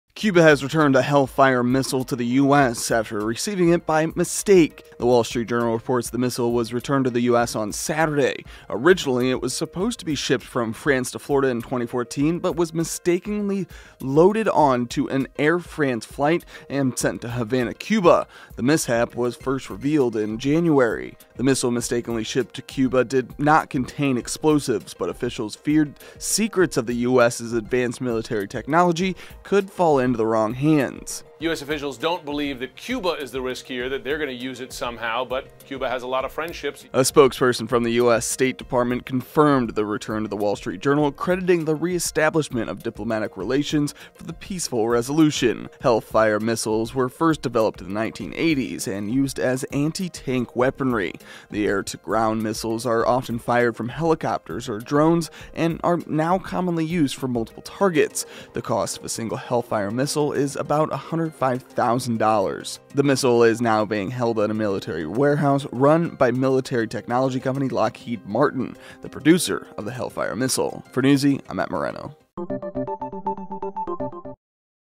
Cuba Returns Hellfire Missile It Mistakenly Received From The US - Newsy
The missile was reportedly shipped from France to Cuba by mistake in 2014. Cuba returned it to Florida on Saturday.

Sources:
US Navy
U.S. Department of Defense

Image via: U.S. Department of Defense / Staff Sgt. Isaac Graham